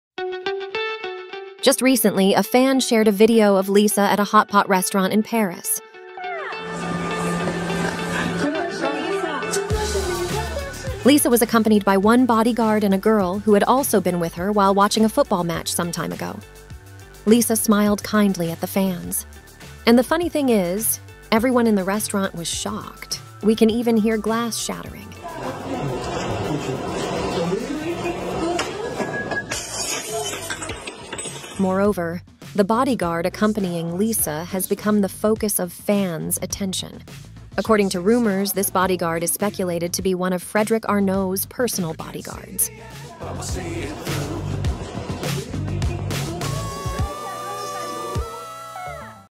Lisa made everyone in the Hot Pot restaurant shocked, even glass shattering!!
Just recently, a fan shared a video of Lisa at a hotpot restaurant in Paris. Lisa was accompanied by one bodyguard and a girl who had also been with her while watching a football match some time ago.

Lisa smiled kindly at the fans. And the funny thing is, everyone in the restaurant was shocked, we can even hear glass shattering.

Moreover, the bodyguard accompanying Lisa has become the focus of fans' attention. According to rumors, this bodyguard is speculated to be one of Frederic Arnault's personal bodyguards.